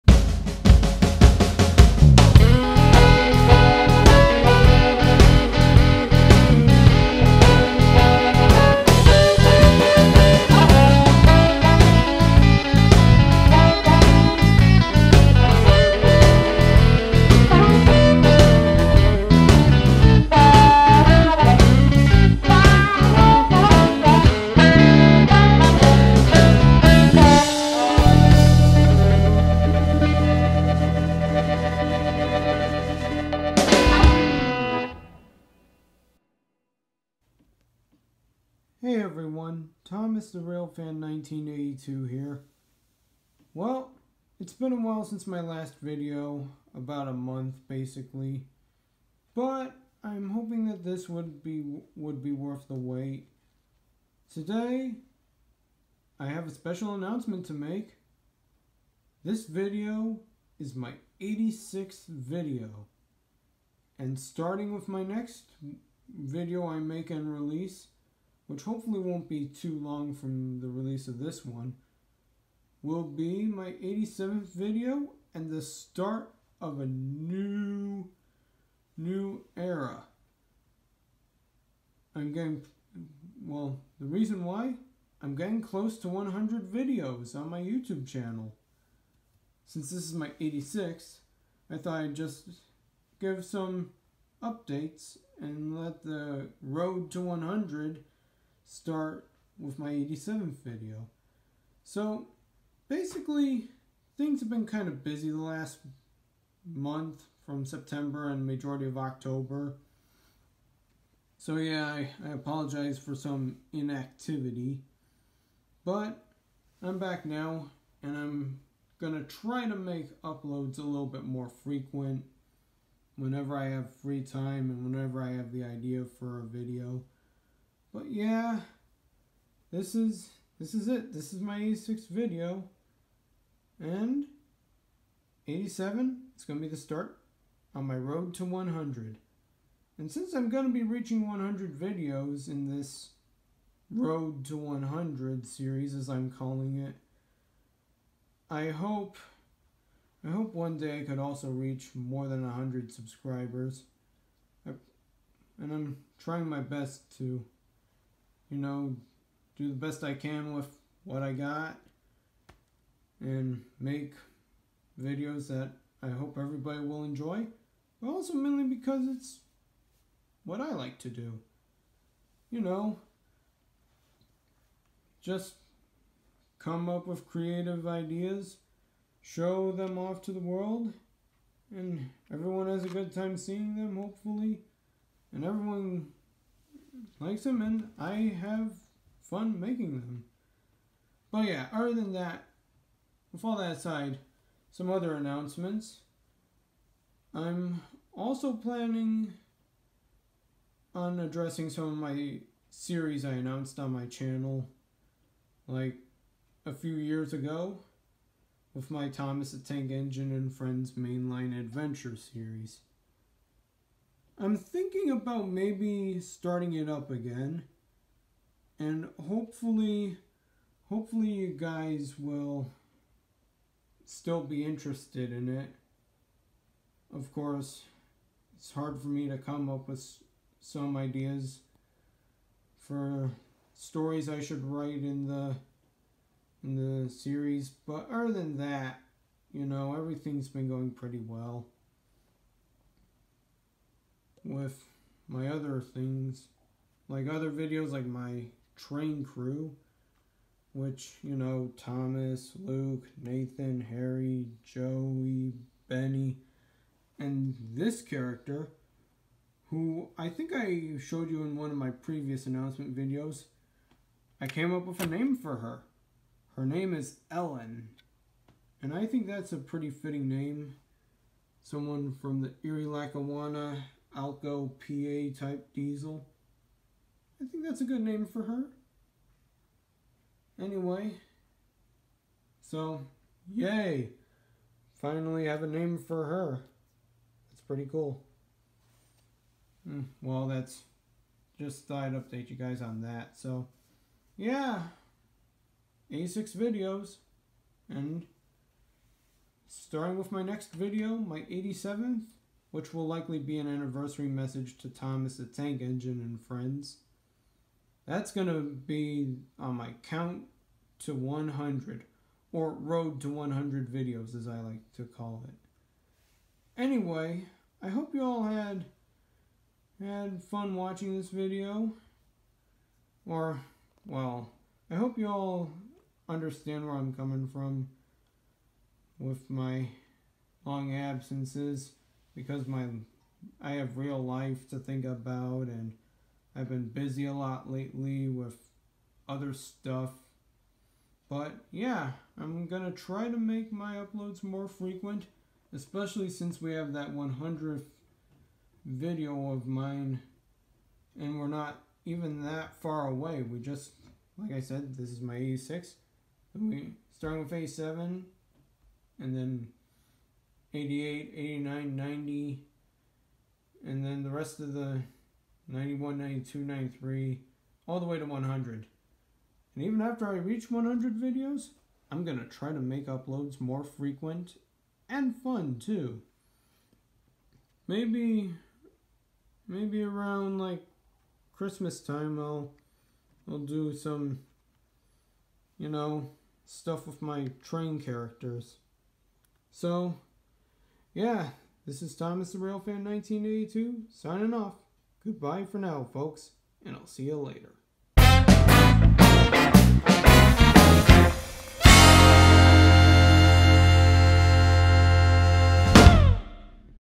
October 2021 update and Special Announcement
This is my 86th video on YouTube. I hope to post more frequently now since my next video, my 87th video, is the start of my countdown to 100 videos which I'm calling my "Road to 100" Enjoy.
  --Thomas the Railfan 1982